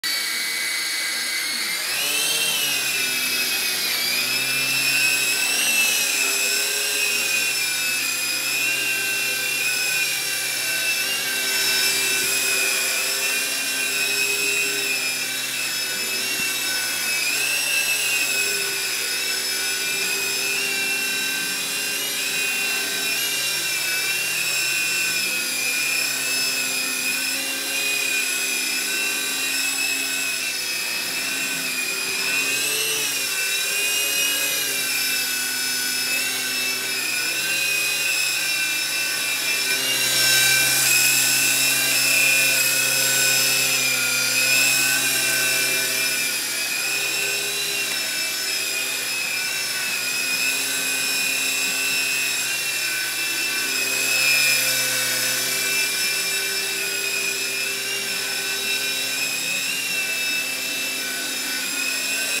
Helico RC Le T642C
Une toute nouvelle génération d'hélicoptères radio commandés. Plus puissant, plus sensible, un design moderne et élegant, cet hélicoptère est en plus doté d'une caméra, qui contrairement aux autre modèles est entiérement mobile, ce qui offre des possibilités infini de réalisation de vidéos. Extrement maniable, stable et agréable à piloter, ce modèle est parfait pour apprendre à piloter mais aussi pour réaliser nombres de figures et videos.